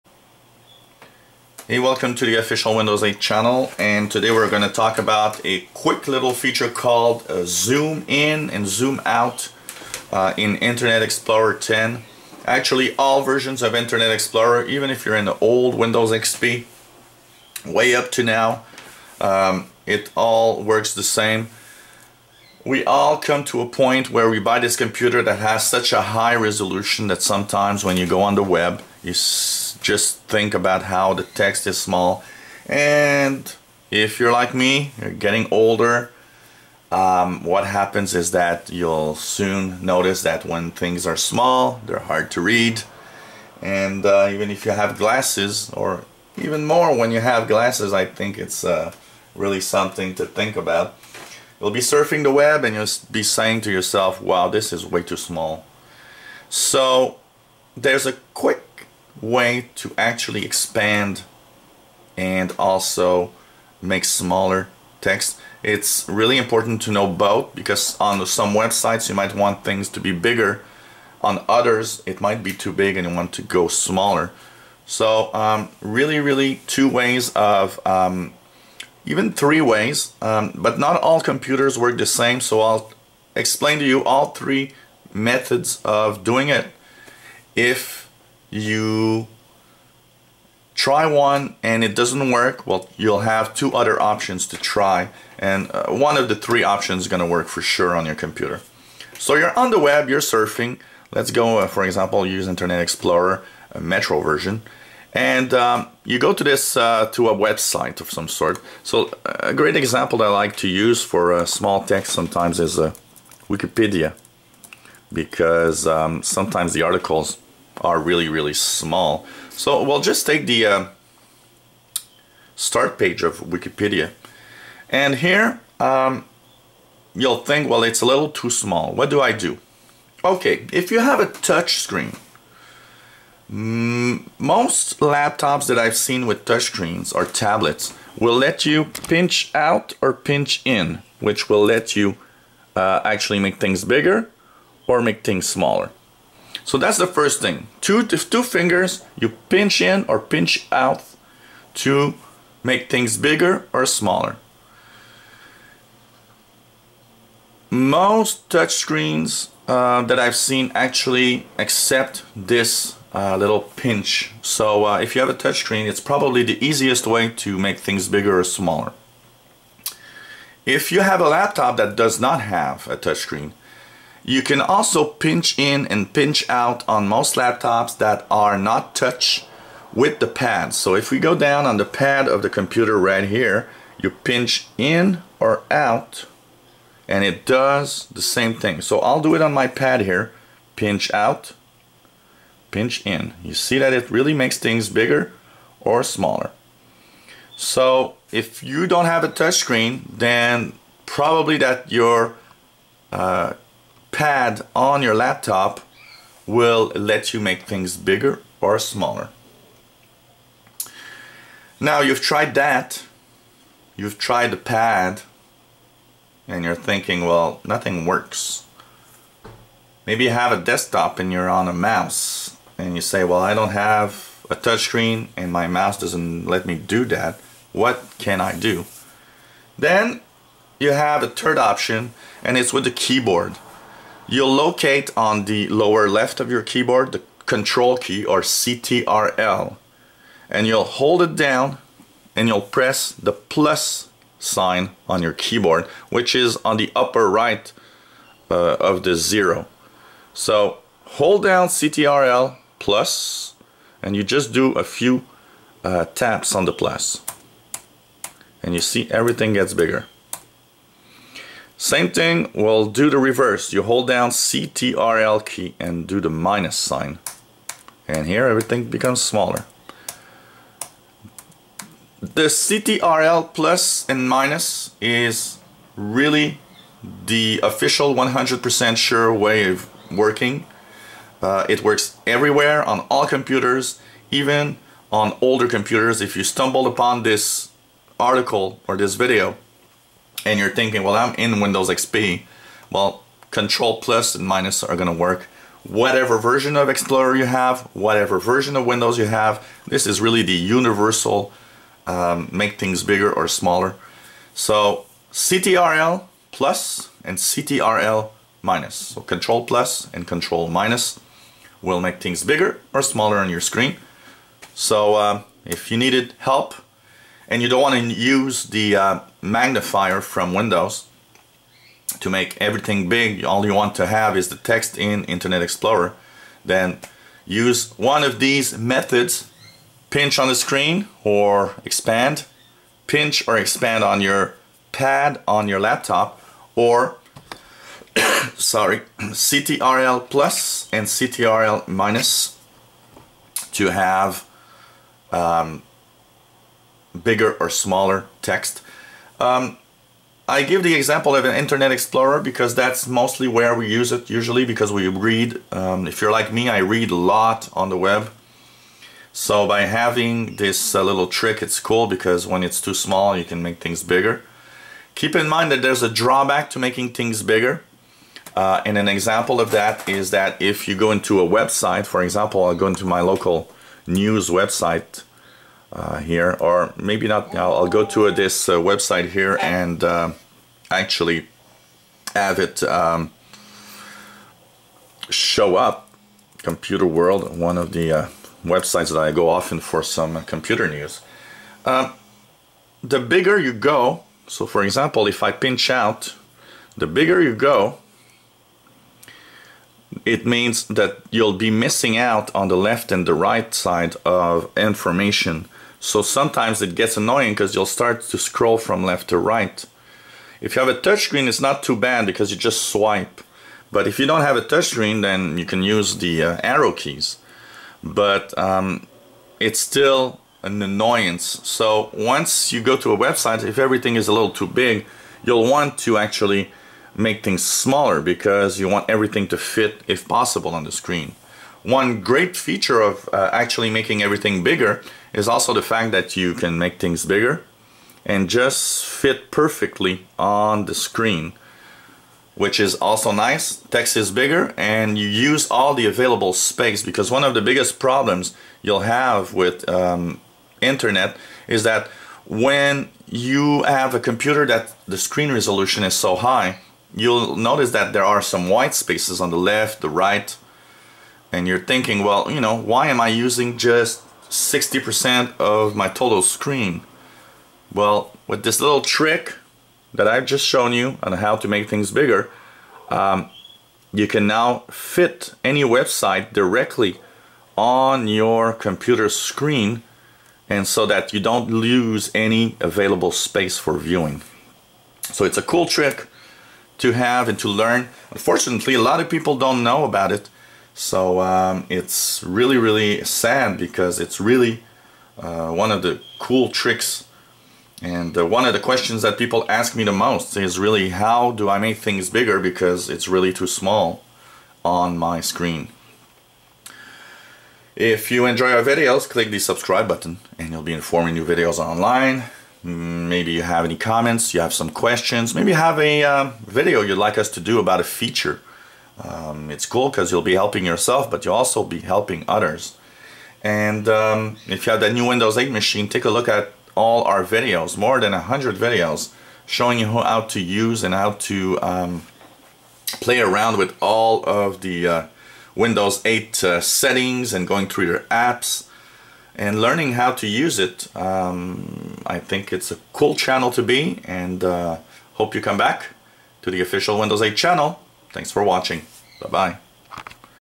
Windows 8 How to zoom in and out of webpages
make everything bigger or smaller on the web with these tricks to zoom in and out in windows 8 internet explorer